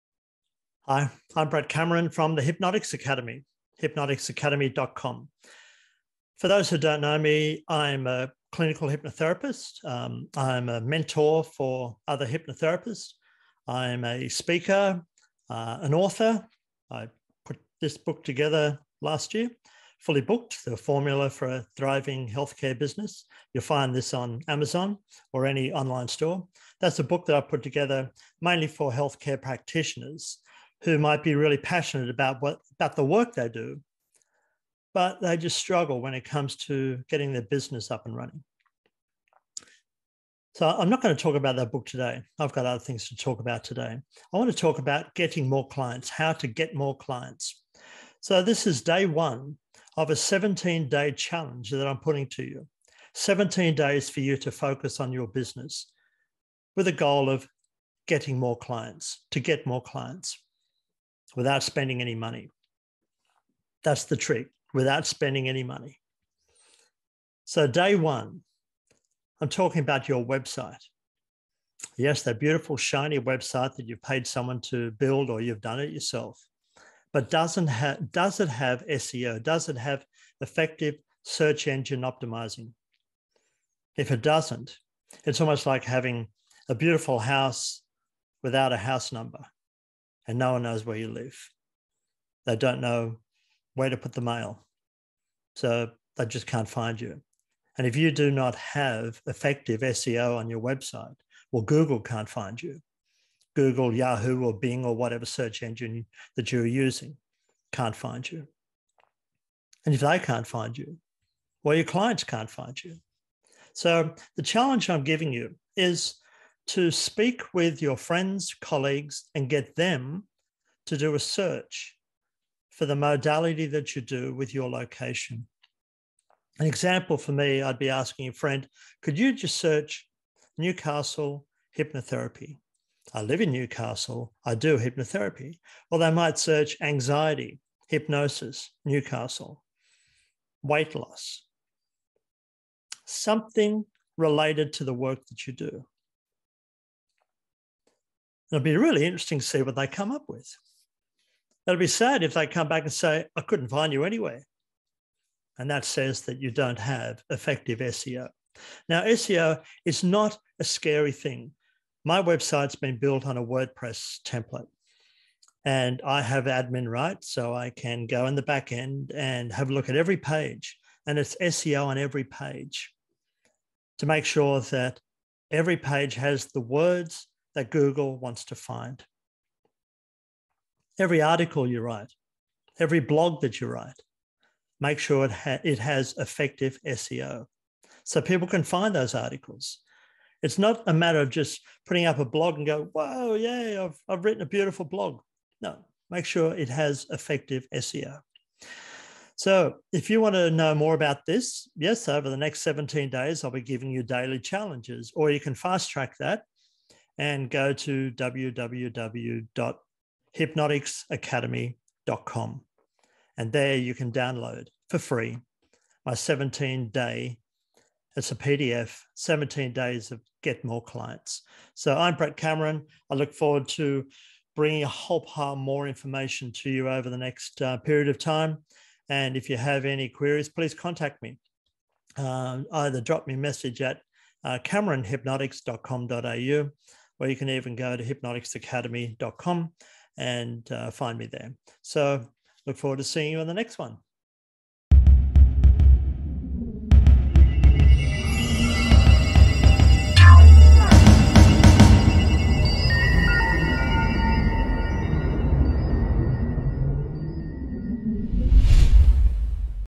Day 1: "Get More Clients" - Your website SEO
You might have the best looking website in the business but if it doesn't have effective Search Engine Optimization (SEO) then no-one will find it. I have created a 17 day "Get More Clients" challenge with a simple business task for you to apply daily.
For the 17 day Get More Clients PDF download go